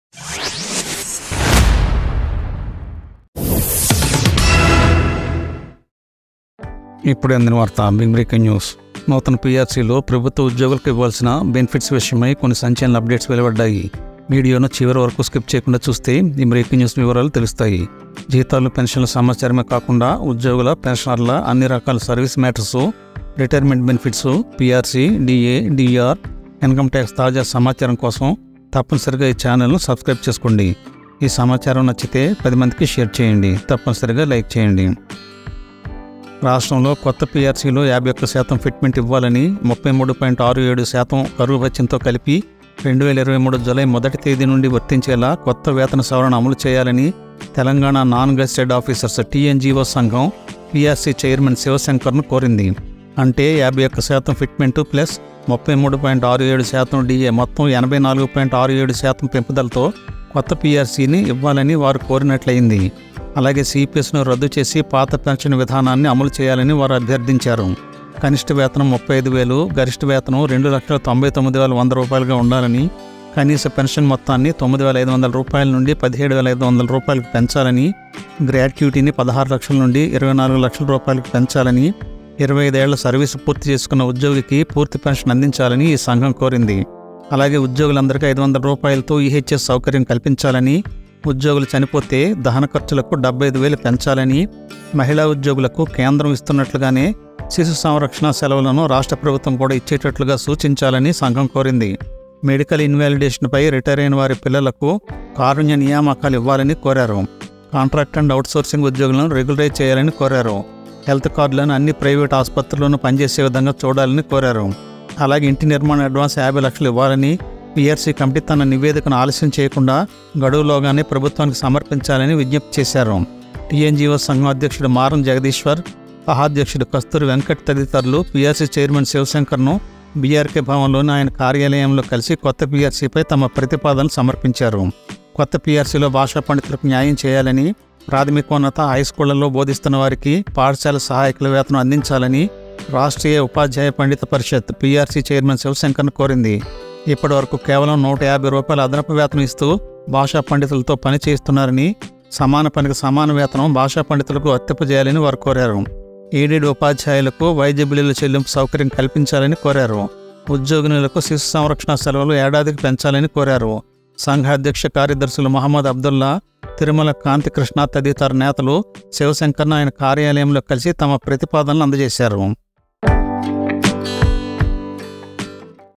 ఉద్యోగులకు-పెన్షనర్లకు సూపర్ గుడ్ న్యూస్-84.67% పెంపుదలతో కొత్త పిఆర్సీ
Description:
The Telangana Non-Gazetted Officers (TNGO) Association has urged PRC Chairman Sivashankar to implement a 51% fitment in the new Pay Revision Commission (PRC) along with a 33.67% dearness allowance (DA) effective from July 1, 2023. They also seek the cancellation of the Contributory Pension Scheme (CPS) in favor of the old pension system.

Key demands include setting the minimum wage at Rs. 35,000, capping the maximum salary at Rs. 2,99,100, and ensuring minimum pension amounts ranging from Rs. 9,500 to Rs. 17,500. Additionally, they propose increasing gratuity from Rs. 16 lakh to Rs. 24 lakh and granting full pension to employees completing 25 years of service.

Further requests encompass raising cremation expenses to Rs. 75,000, aligning childcare leave provisions with those of the central government, and facilitating compassionate appointments for the children of medically invalidated retirees. The association also advocates for the regularization of outsourced employees, functional health cards in all private hospitals, and a house construction advance of Rs. 50 lakhs.

Meanwhile, the Rastriya Upadhyaya Pandita Parishad emphasizes the need for justice for linguists in the new PRC, advocating for school assistants' wages for those teaching in primary and high schools. They advocate for equal pay for equal work for language scholars and urge the provision of medical bill payment facilities for aided teachers, along with an extension of childcare leave to one year.

These proposals were presented by TNGO Association President Maram Jagadeeswar, Co-President Kasturi Venkat, and others, while the Rastriya Upadhyaya Pandita Parishad's demands were communicated by Association President Mohammed Abdullah, Secretary Tirumala Kanti Krishna, and other leaders in meetings with PRC Chairman Sivashankar at BRK Bhavan.

11.  Music from YouTube music library
12.  Music from Video Editors used in the video making